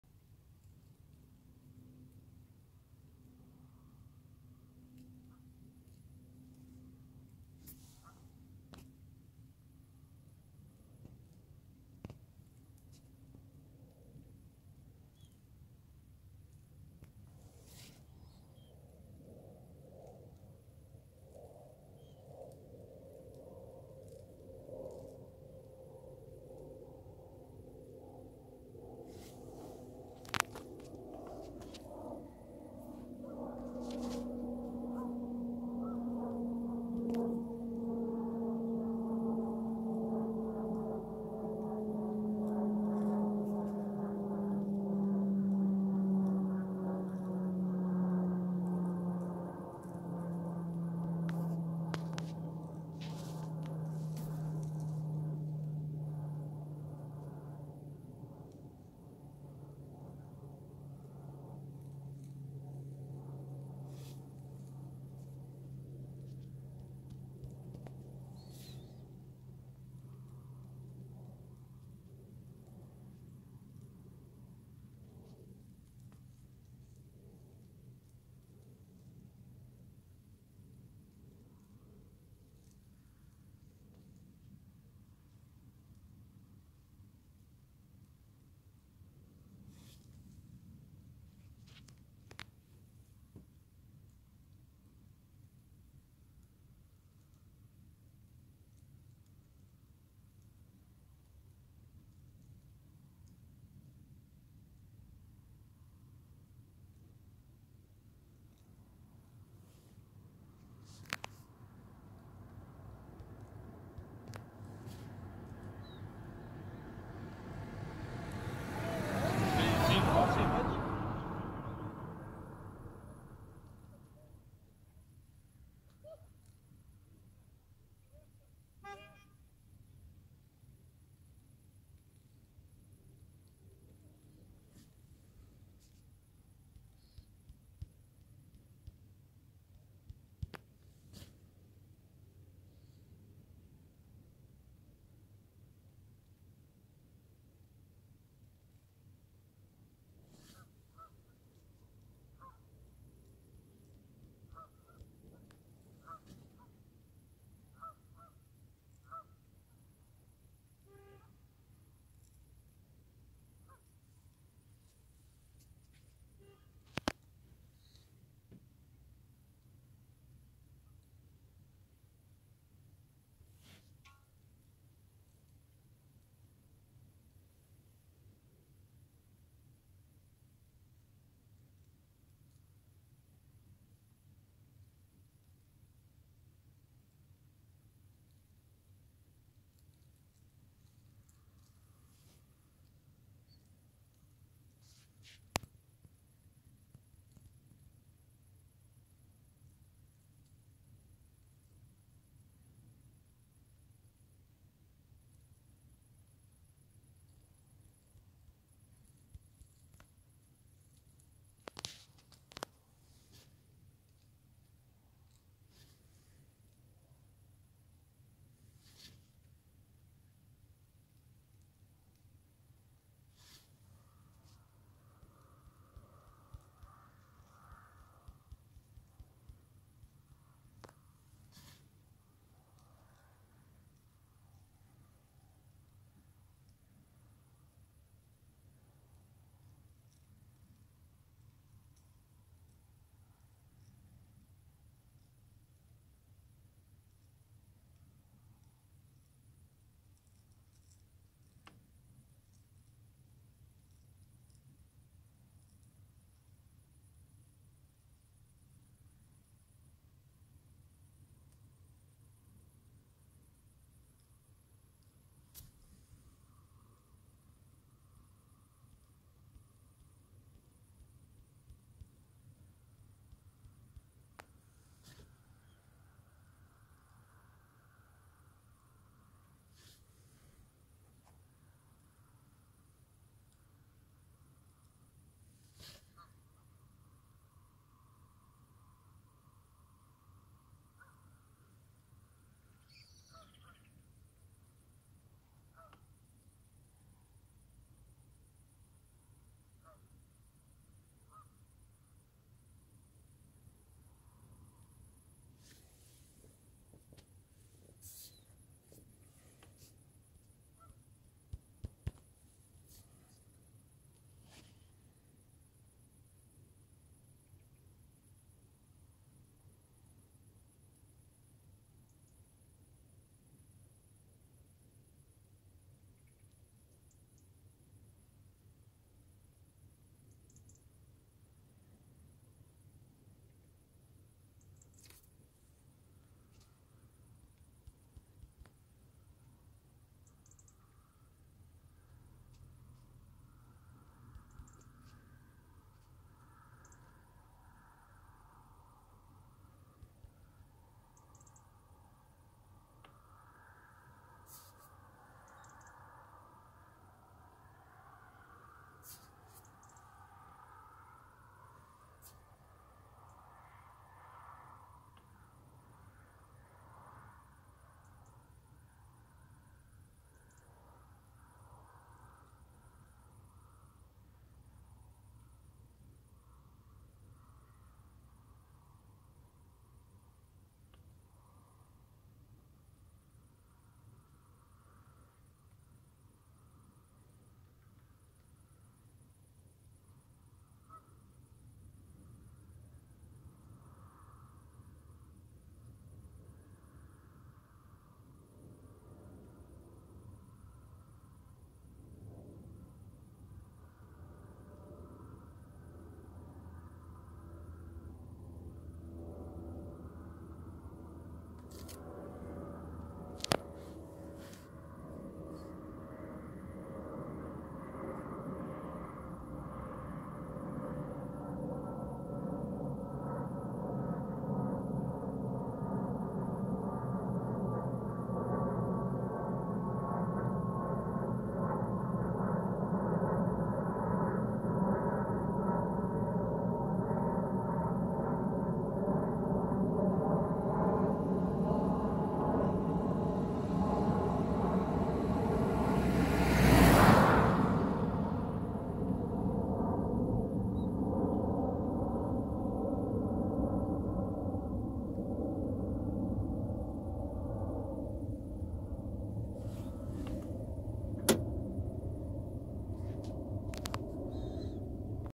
DES OIES SAUVAGES SE REPOSENT, POUR LE GRAND VOYAGE EN AFRIQUE !!
DES OIES SAUVAGES SE REPOSENT AVANT DE PARTIR EN AFRIQUE. 05/08/2020. 21H00.